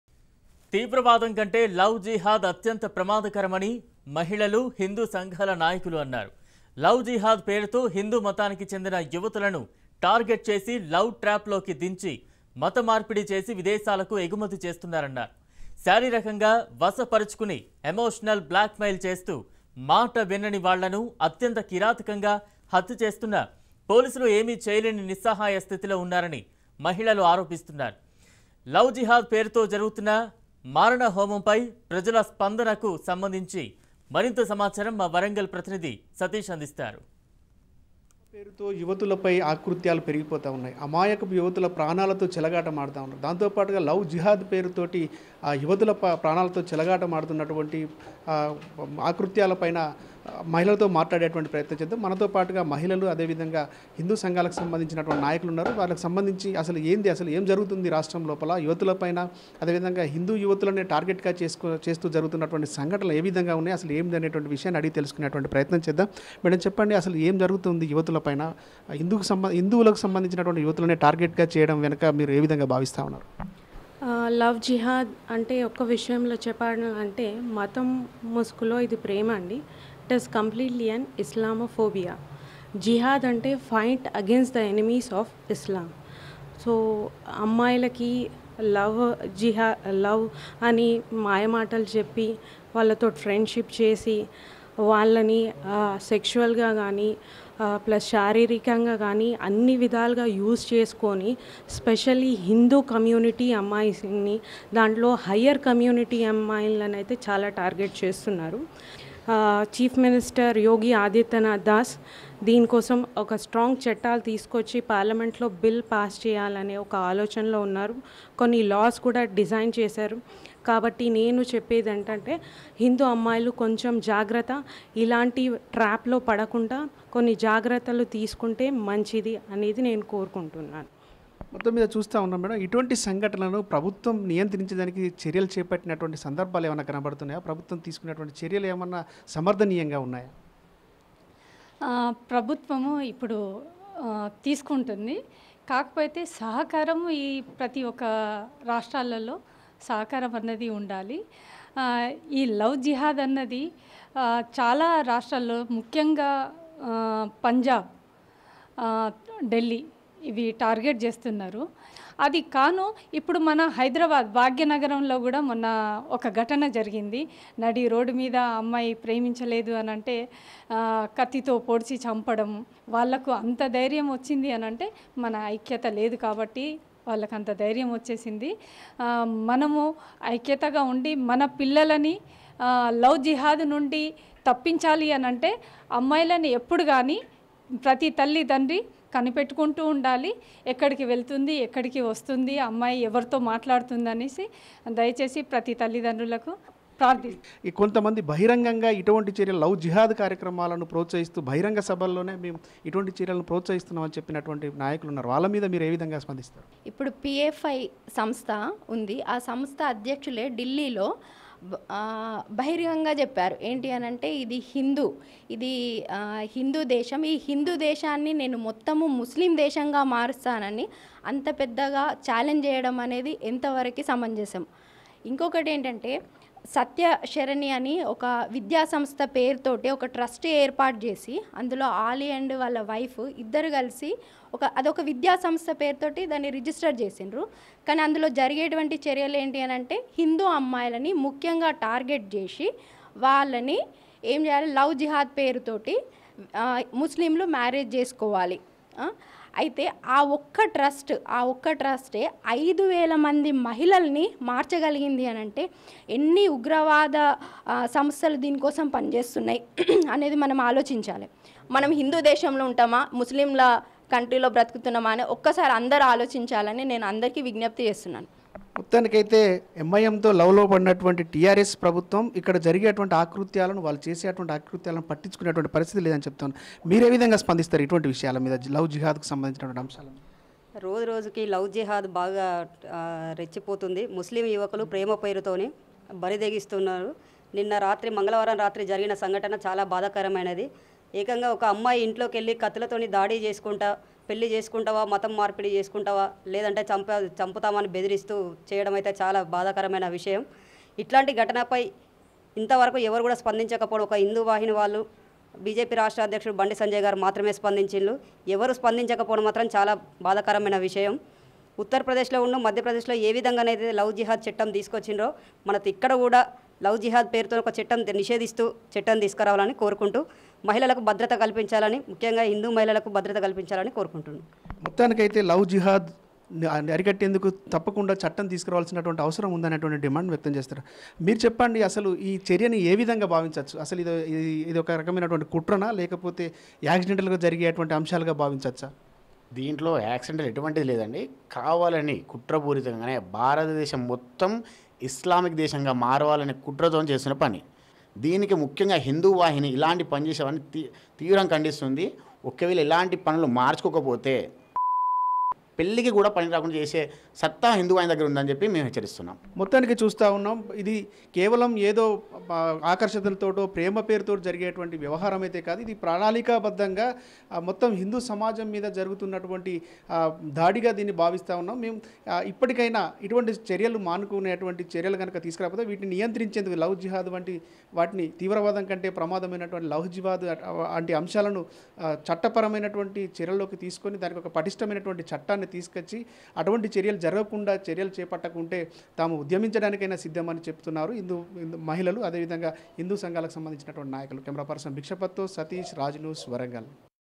లవ్ జిహాద్ పై మహిళలు, హిందూ సంఘాలు ఆగ్రహం | Warangal Womens Reaction on Love Jihad | Raj News
లవ్ జిహాద్ పై మహిళలు, హిందూ సంఘాలు ఆగ్రహం | Warangal Womens Reaction on Love Jihad | Raj News Telugu